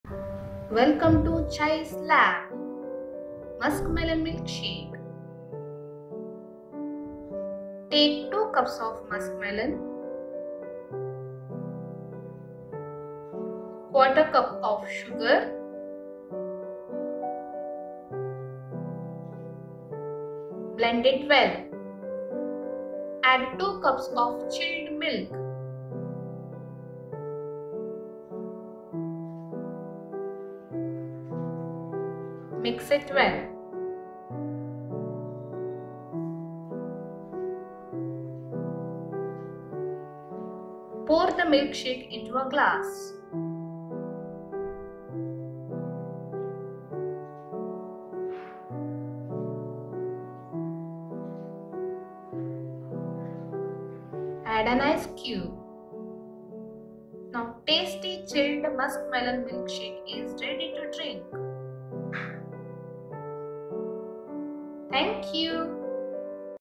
Muskmelon Milkshake | Easy Milkshake | Quick Milkshake | Beverage | Healthy Fruit Juice | Recipe
To Make Muskmelon Milkshake:

Muskmelon - 2 Cups
Sugar - 1/4 Cup
Chilled Milk - 2 Cups
Ice Cubes